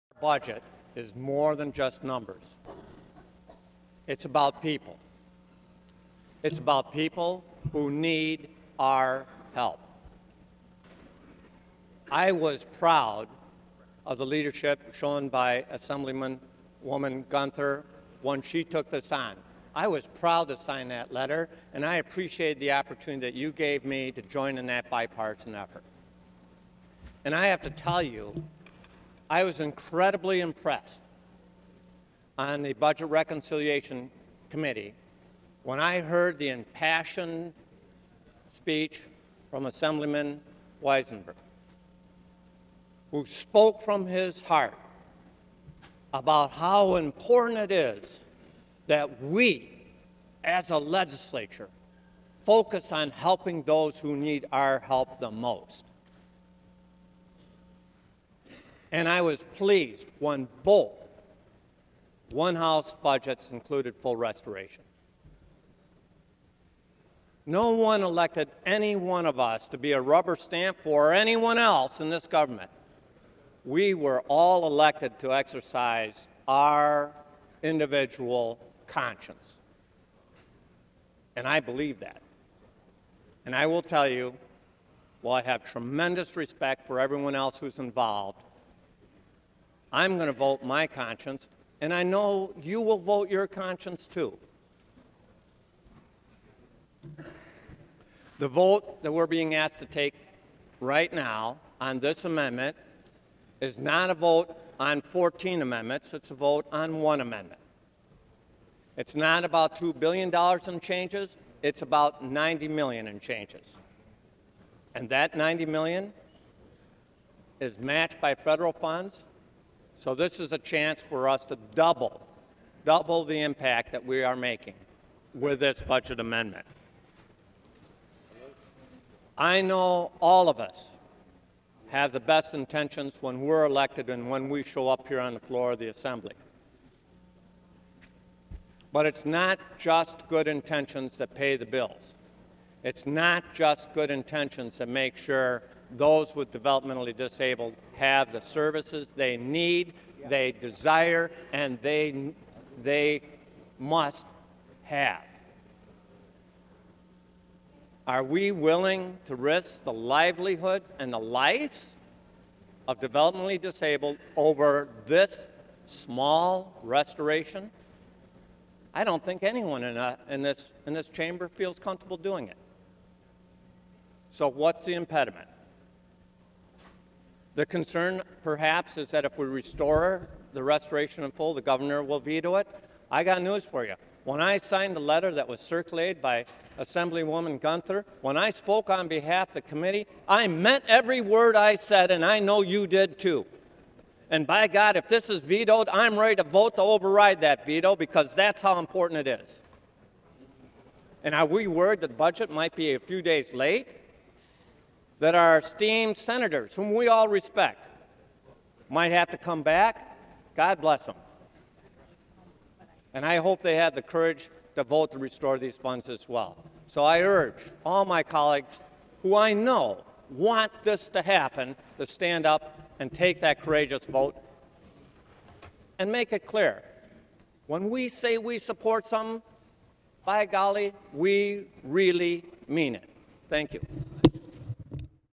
NYS ASSEMBLYMAN ANDY GOODELL URGES FULL FUNDING FOR OFFICE OF PEOPLE WITH DEVELOPMENTAL DISABLITIES
Assemblyman Andy Goodell (R,C,I- Chautauqua) offers a passionate plea for the funding of the Office of People with Developmental Disablities in the 2013-14 Budget.